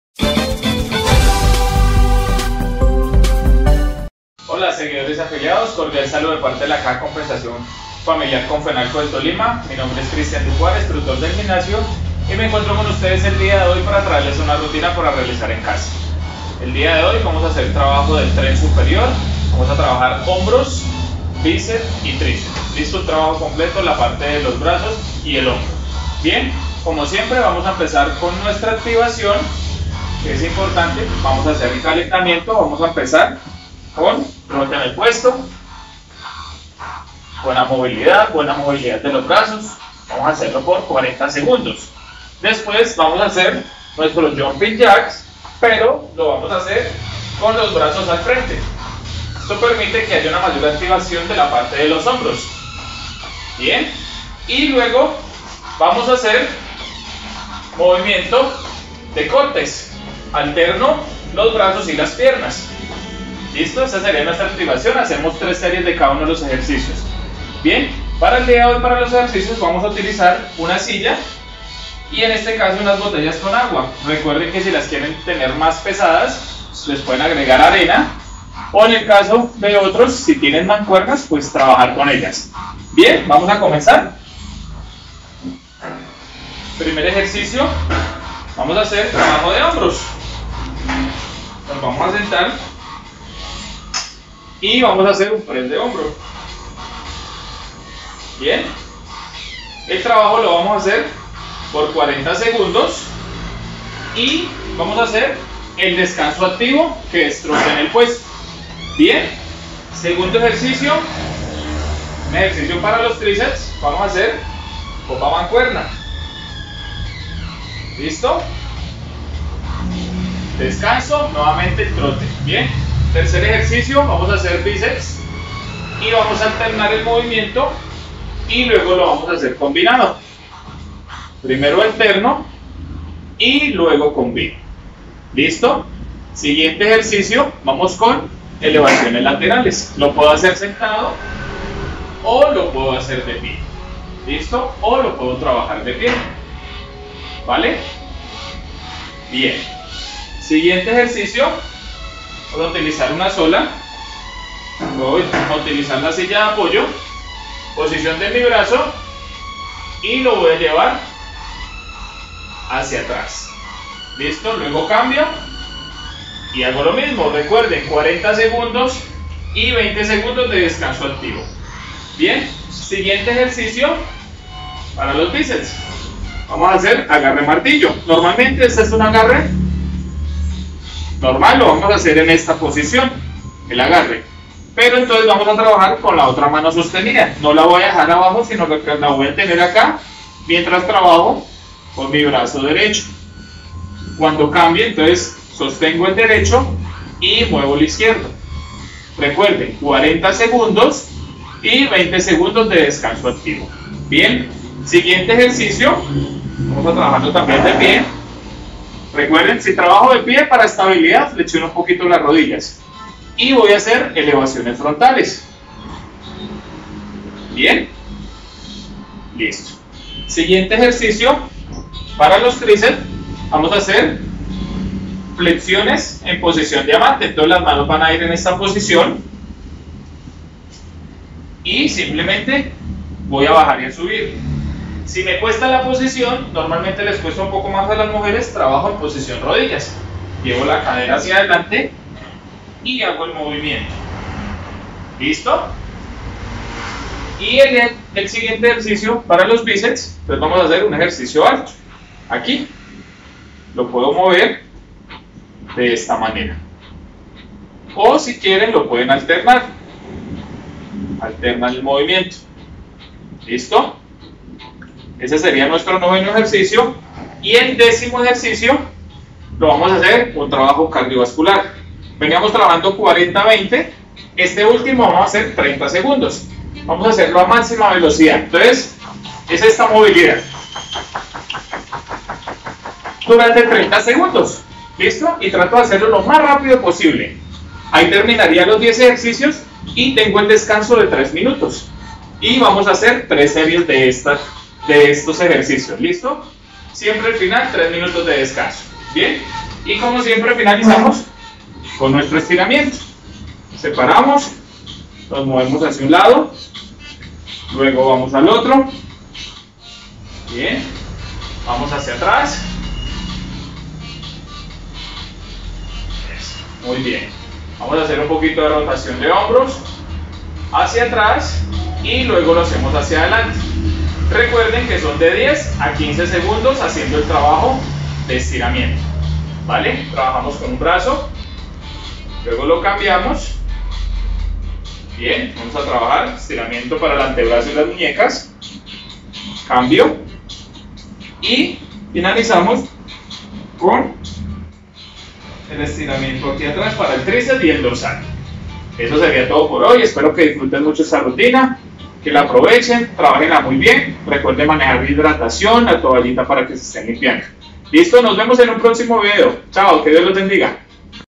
Ejercítate. Complementa la sesión de entrenamiento en casa.
Ejercítate. Complementa la sesión de entrenamiento en casa. Fortalece hombros, bíceps y tríceps